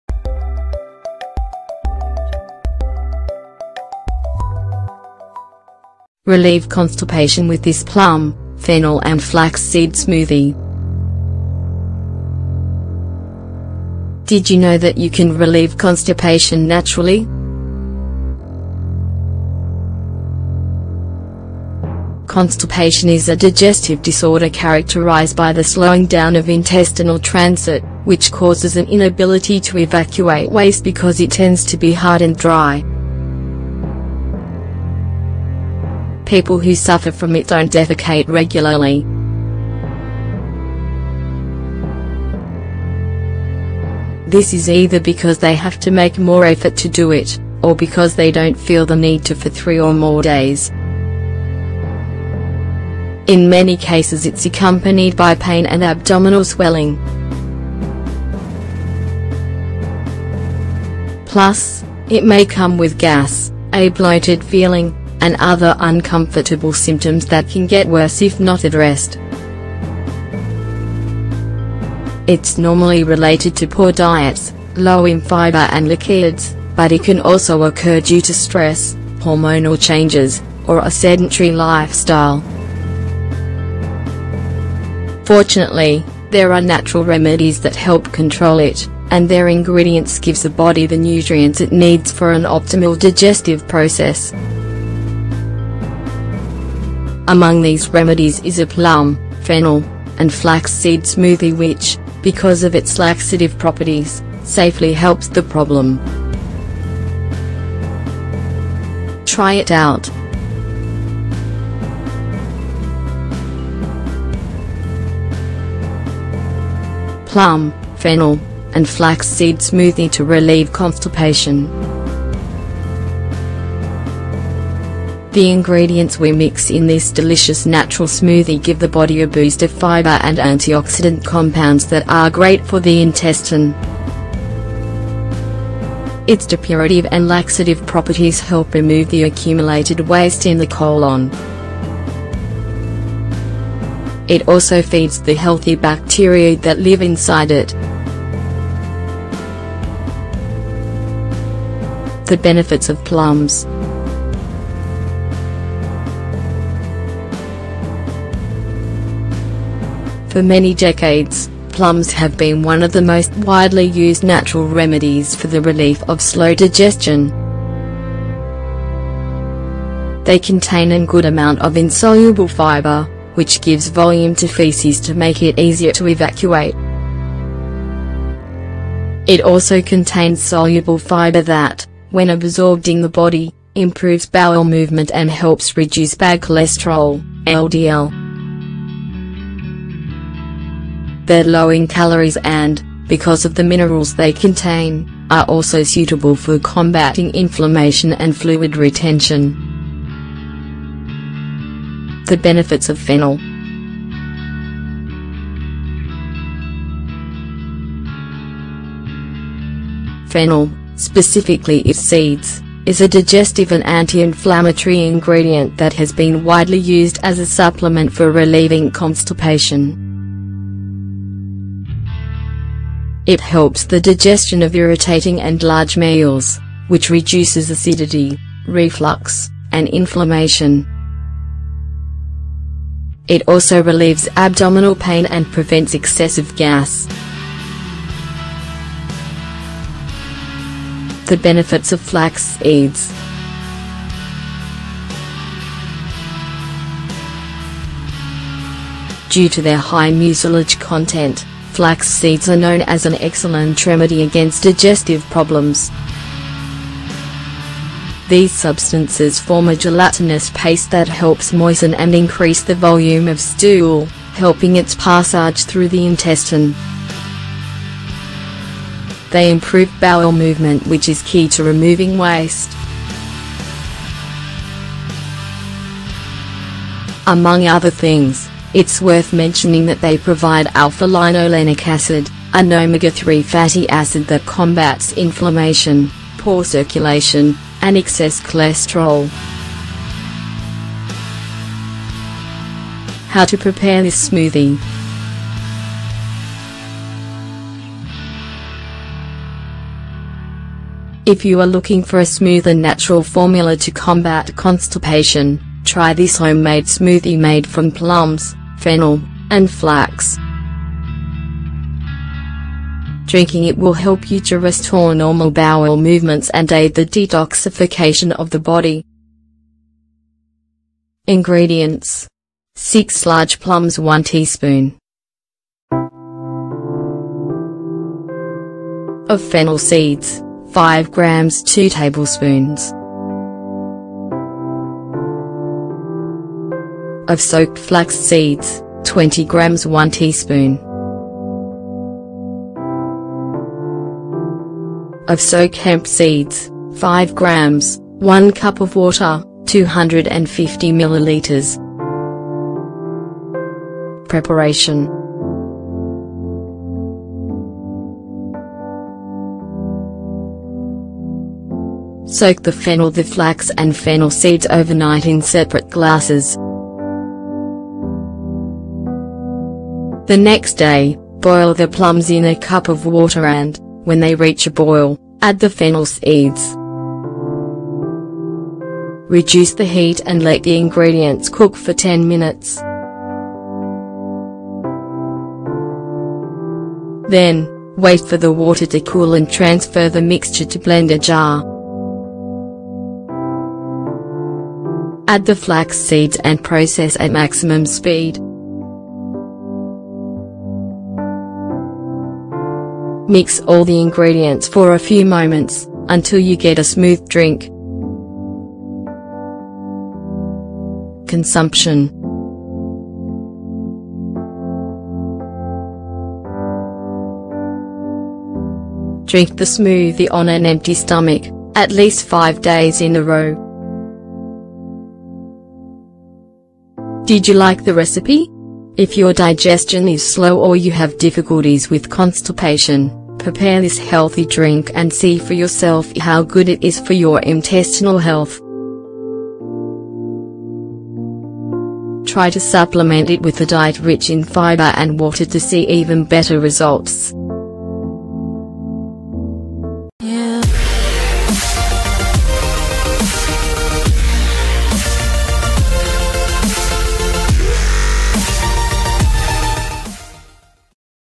Relieve Constipation with this Plum, Fennel and Flax Seed Smoothie - Australia 365
Thanks to the fibre from its ingredients, this smoothie is ideal for relieving constipation and associated discomfort, such as abdominal swelling or gas.
Health Tips